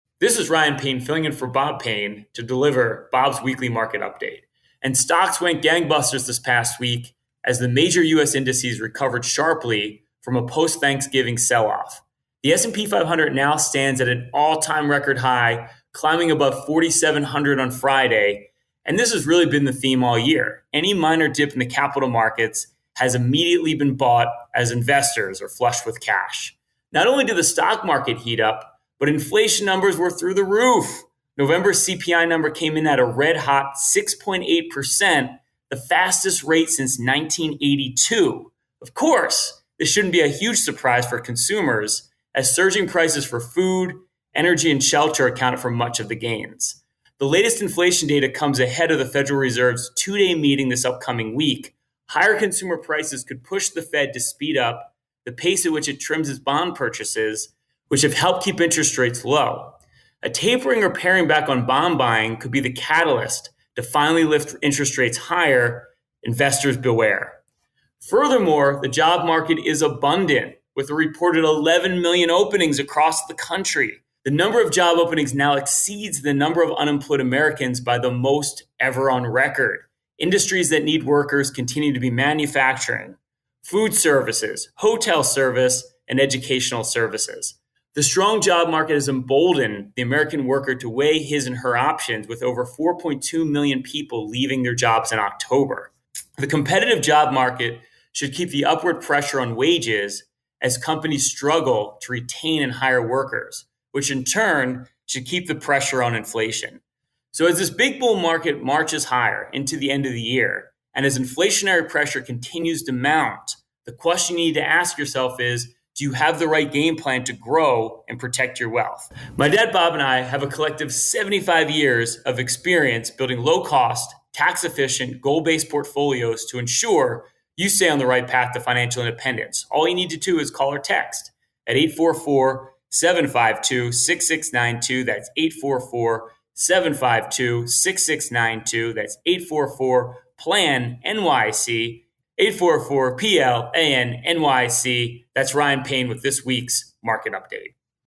The major US indices recovered sharply from a post-thanksgiving sell-off.
Market Update for 12/11 with Ryan Payne